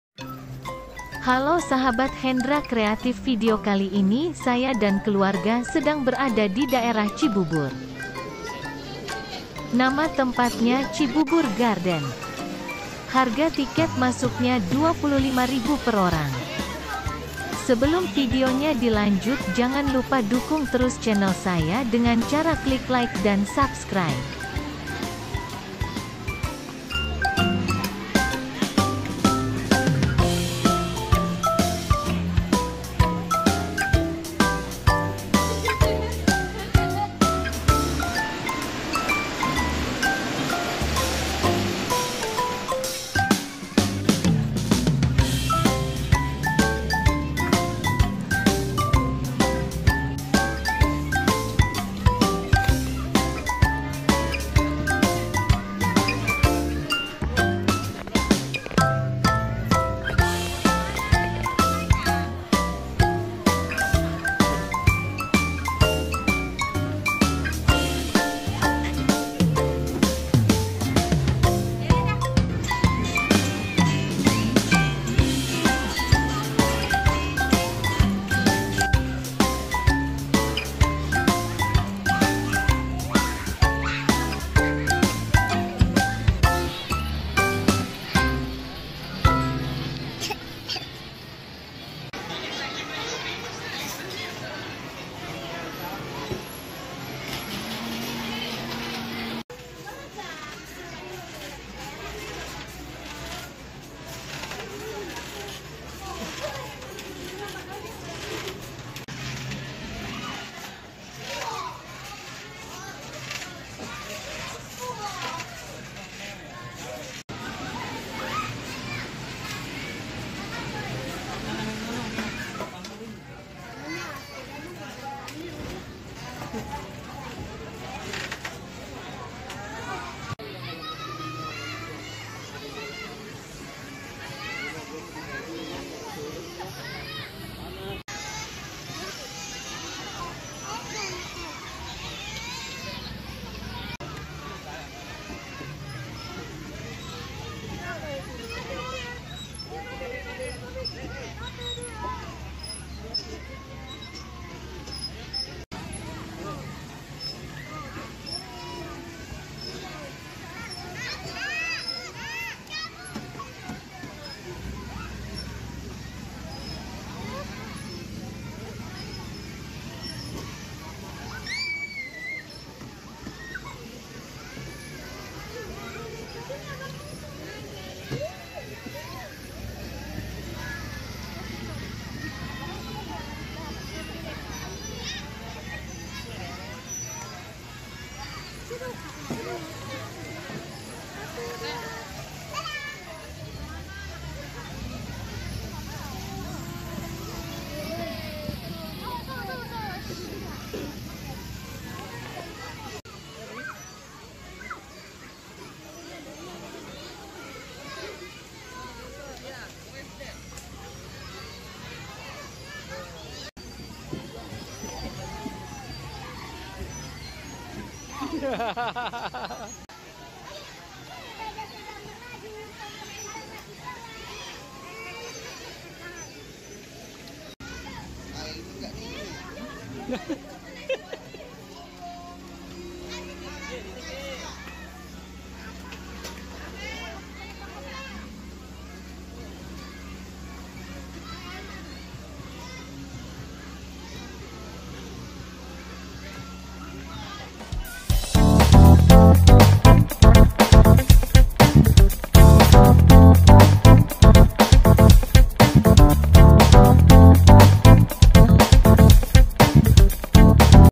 Cibubur garden || eat and play
hallo sahabat hendra kreatif di video kali ini saya dan keluarga sedang berada di daerah cibubur nama tempatnya cibubur garden
lokasinya di belakang rumah makan nyi iteung cileungsi